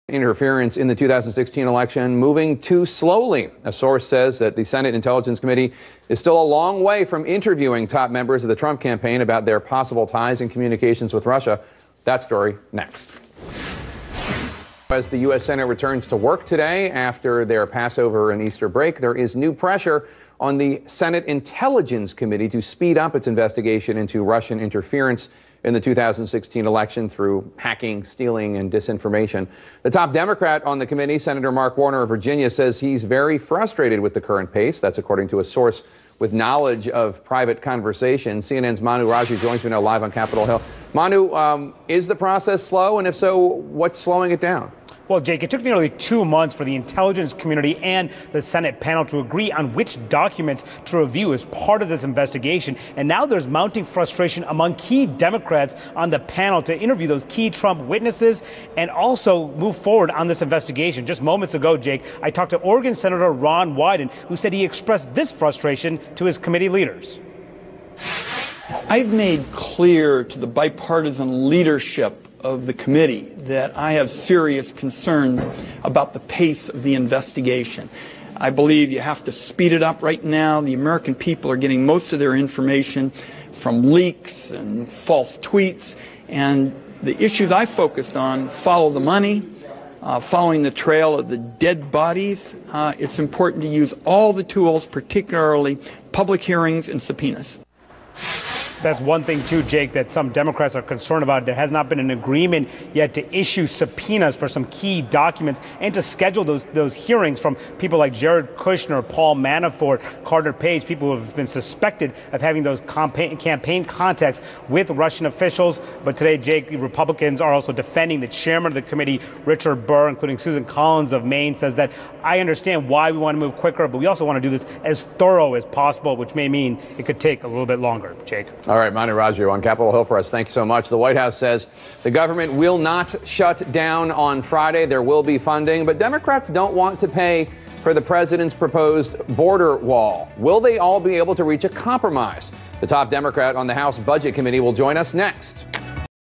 Pressure on Senate Committee To Speed up Russia Probe.
The First 100 Days of President Donald Trump.